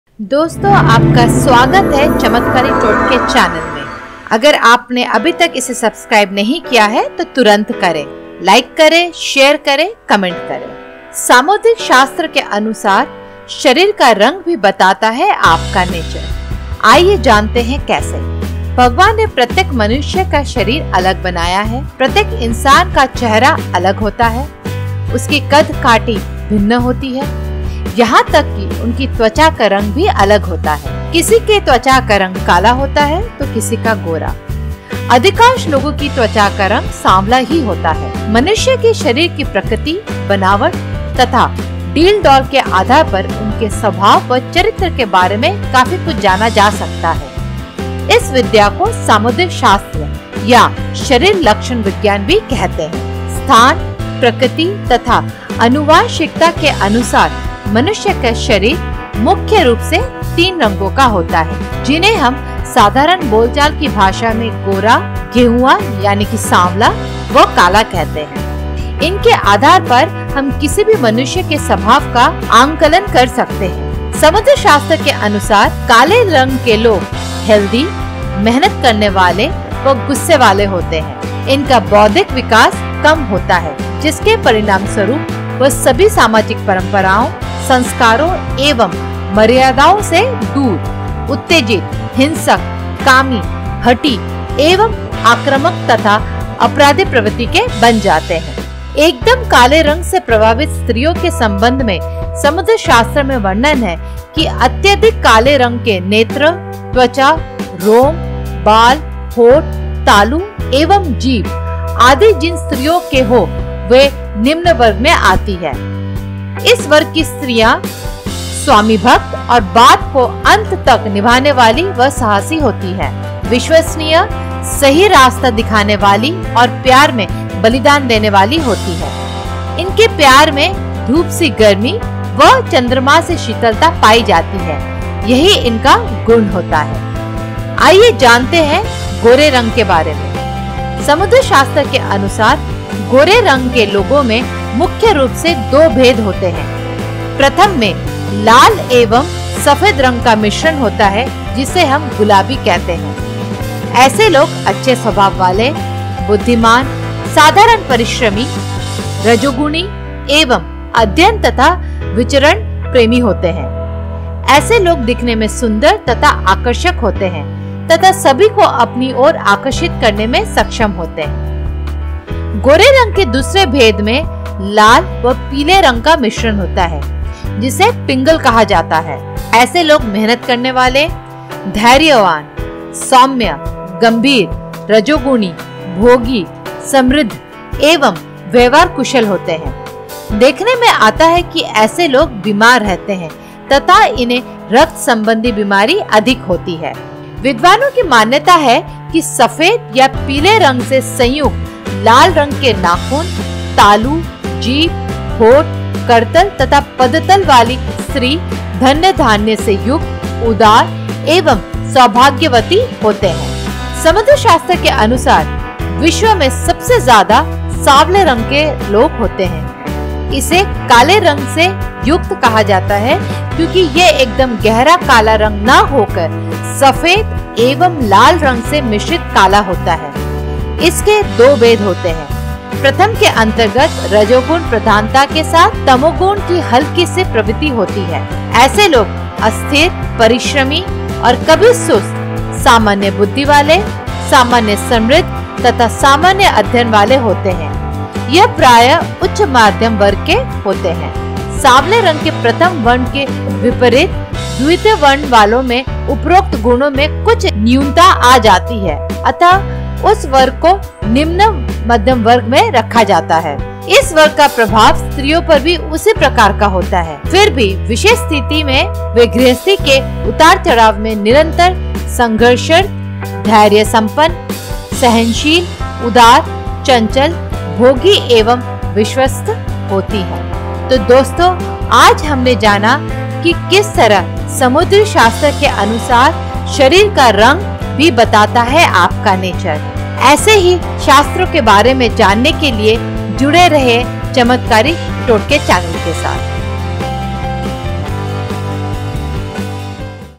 सामुद्रिक शास्त्र के अनुसार शरीर का रंग भी बताता है आपका नेचर Nature according to skin tone
सामुद्रिक शास्त्र के अनुसार शरीर का रंग भी बताता है आपका नेचर According to Samudrik Shastra, the color of the body also tells your nature